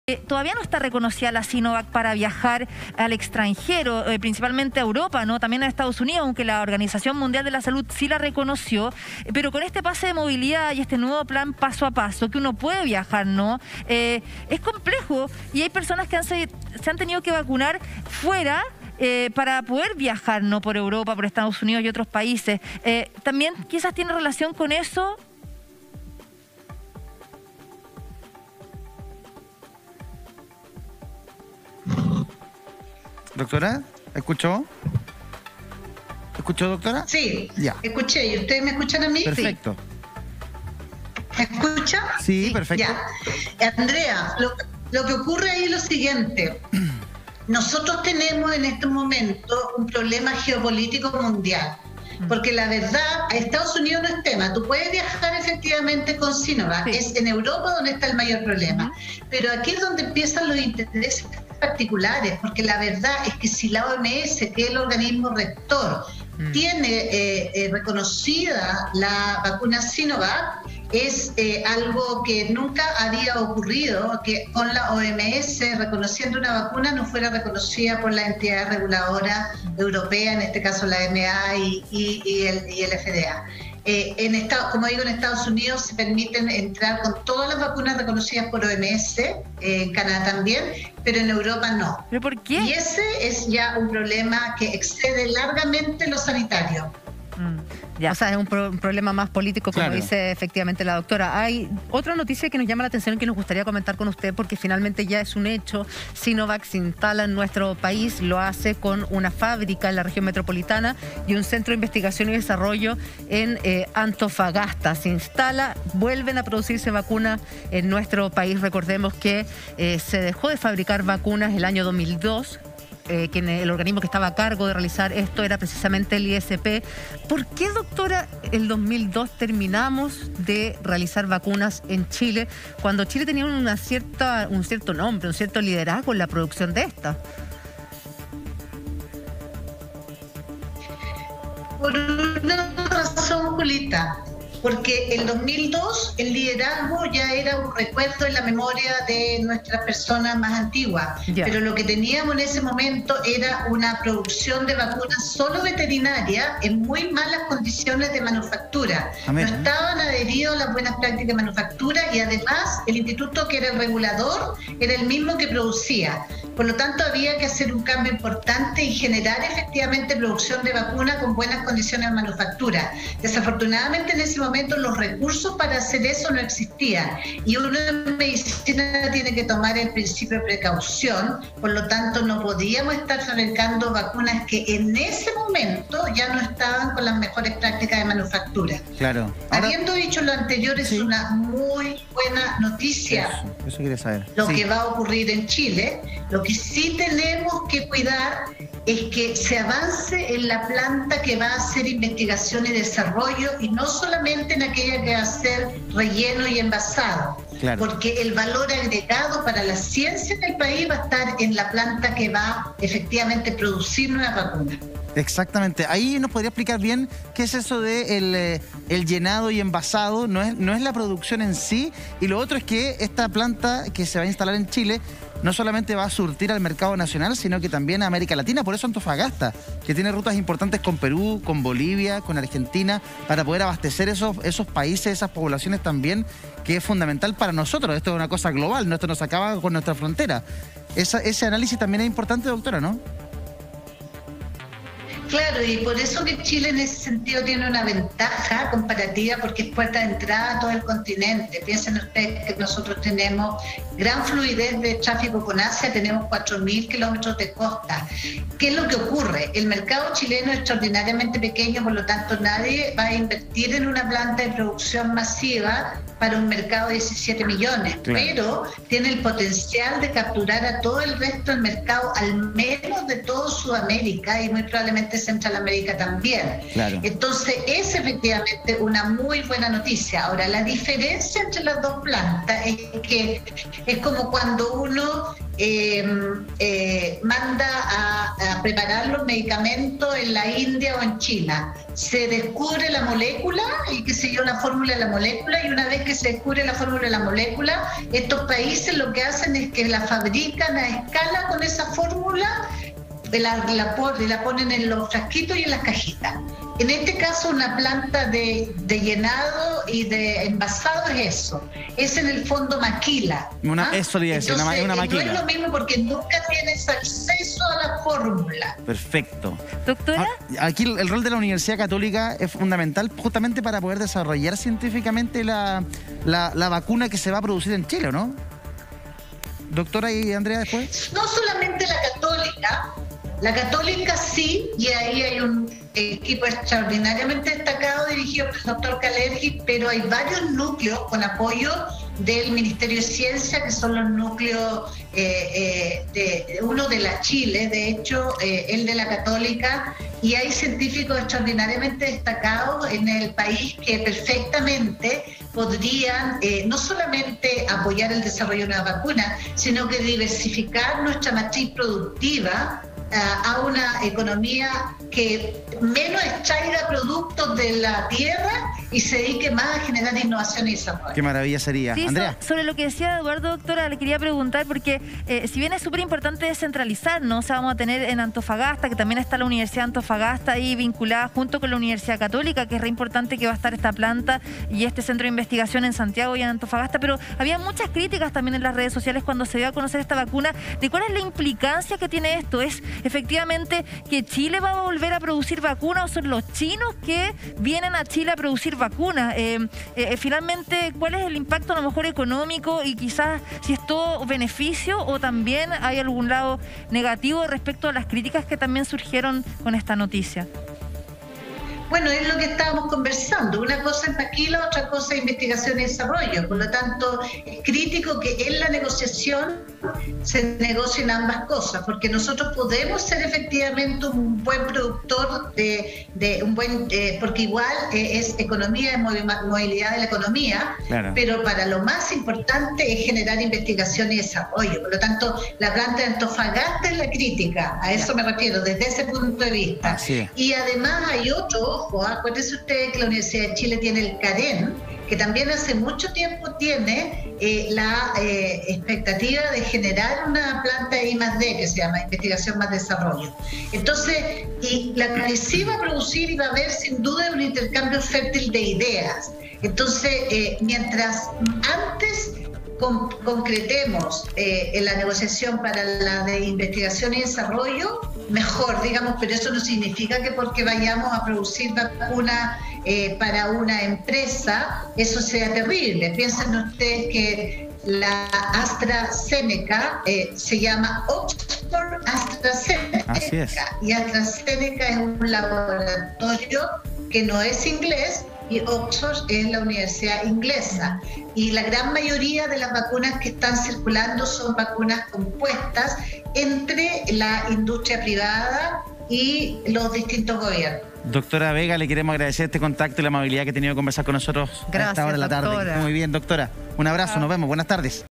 Planta de Sinovac en Chile comenzaría a producir vacunas a partir del 2022
Te entregamos todos los antecedentes de la instalación de la planta del laboratorio que produce la vacuna Coronavac en nuestro país.